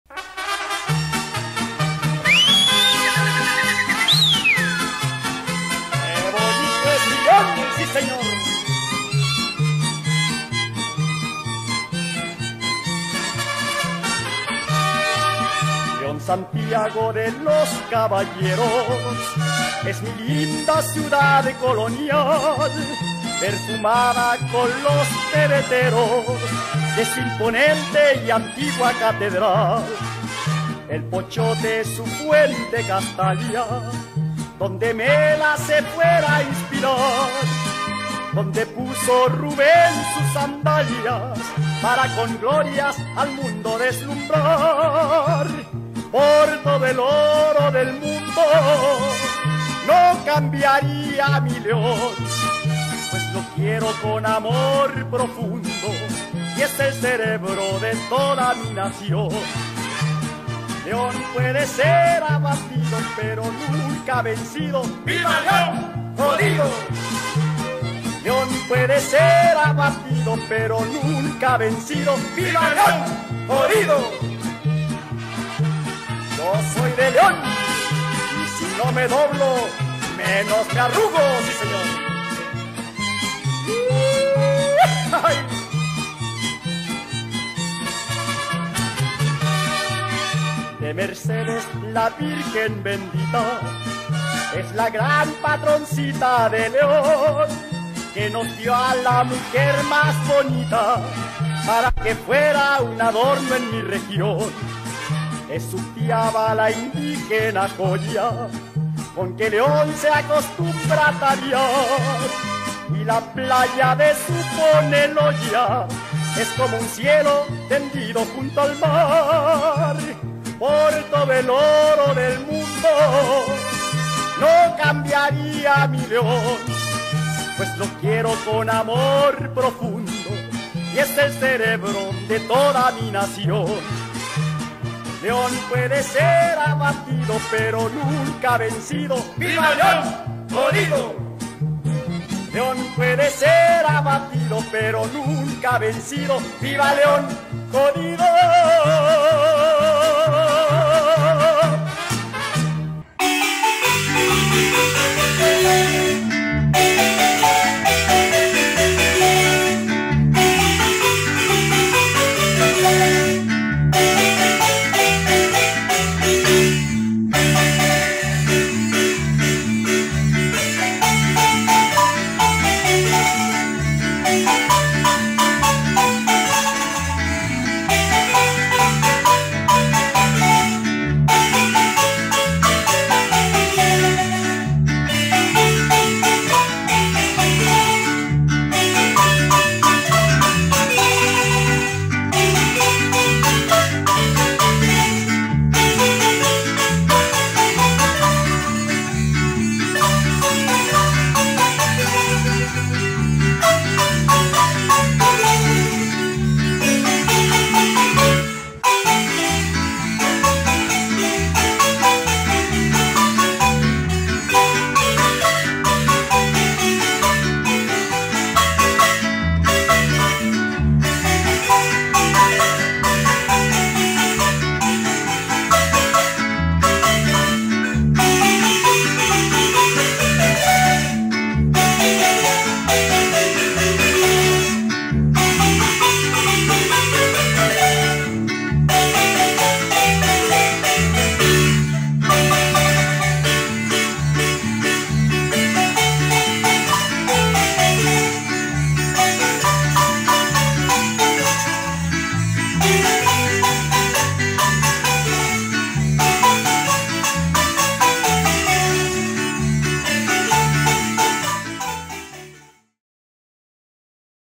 Festival Nacional de la Apicultura
Así se vivió el primer día del Festival Nacional de la Apicultura desde el Sauce, León